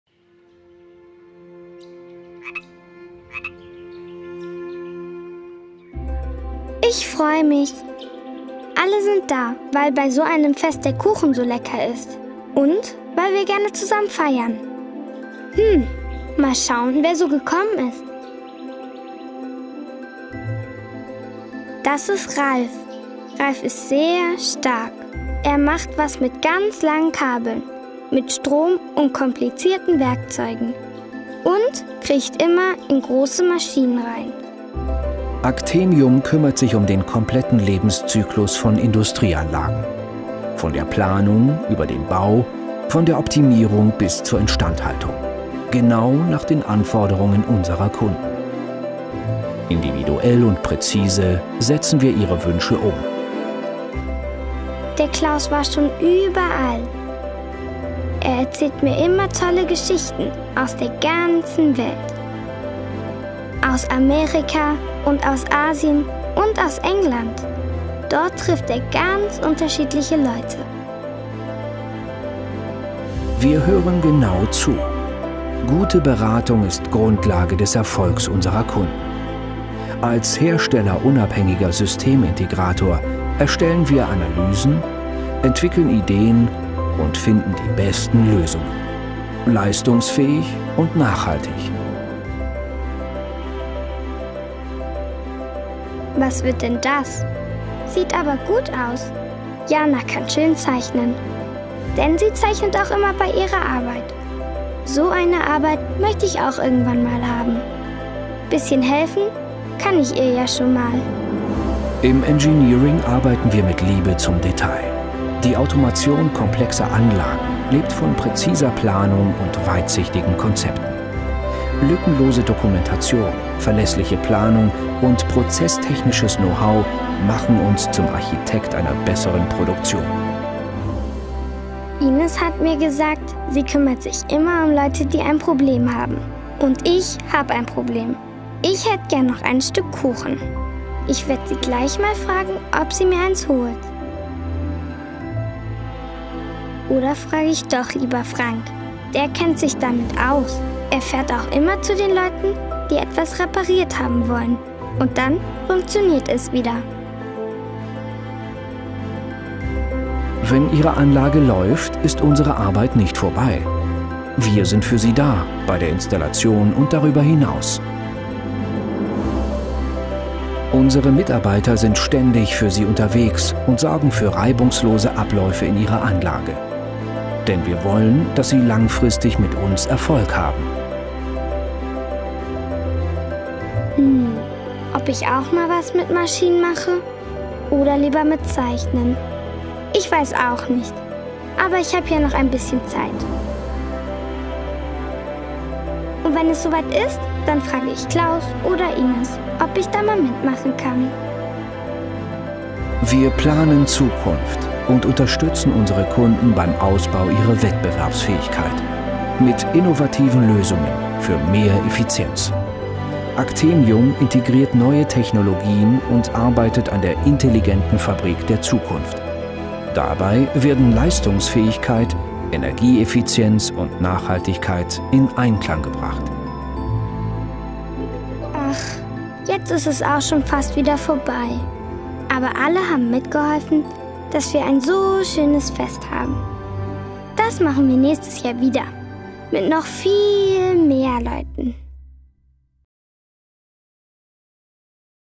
Actemium Imagefilm
Actemium ist die VINCI Energies Marke für die Industrie.

Actemium kümmert sich um den kompletten Lebenszyklus von Industrieanlagen - von der Planung über den Bau, von der Optimierung bis zur Instandhaltung. Genau nach den Anforderungen unserer Kunden. Individuell und präzise setzen wir Ihre Wünsche um.